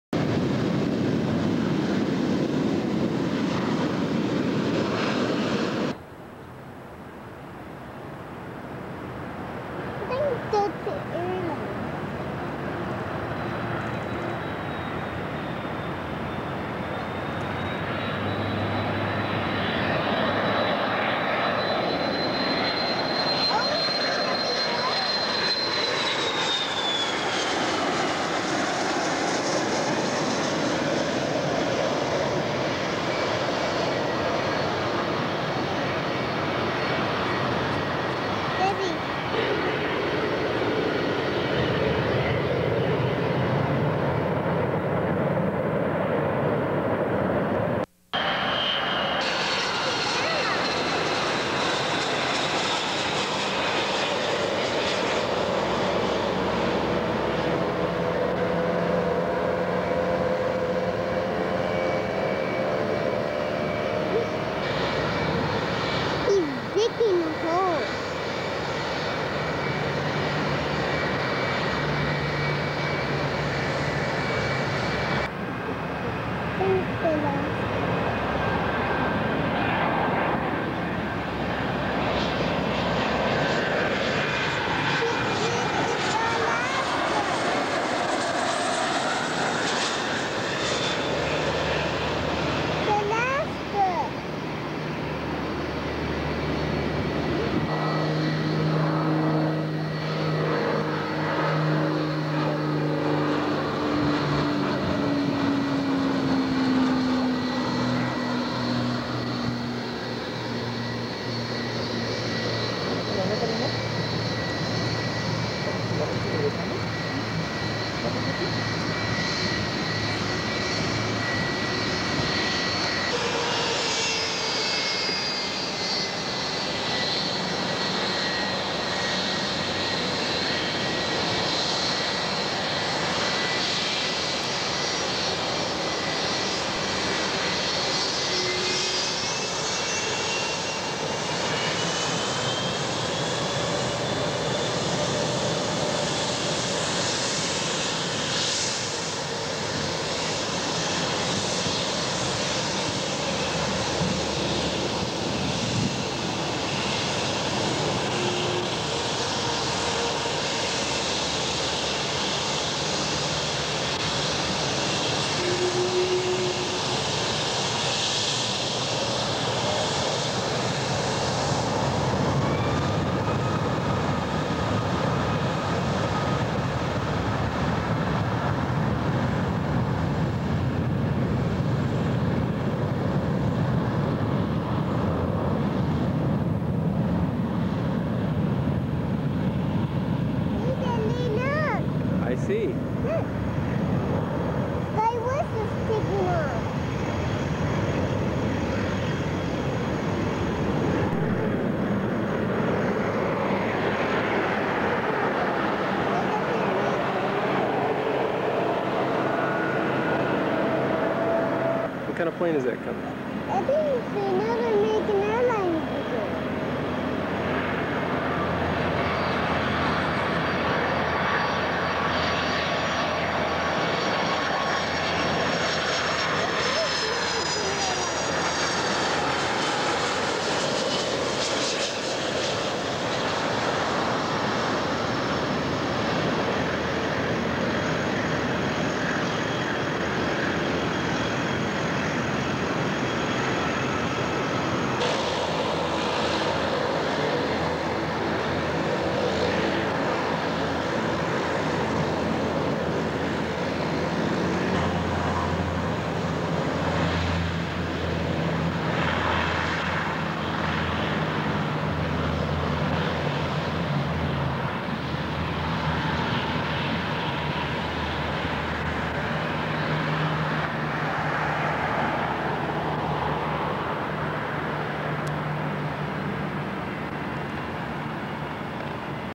John Wayne Airport: NOV '98
Sorry for the little boy yellin, dats me :3 hehe. One things for sure, 90's vids of KSNA aren't common! These the best we gots here, original VHS rip, well, actually, i still got the video camera this was recorded on, it still woks, somethings broken in it so it has more noise and if its too dark you cant see anything, but it still works and the tape works too.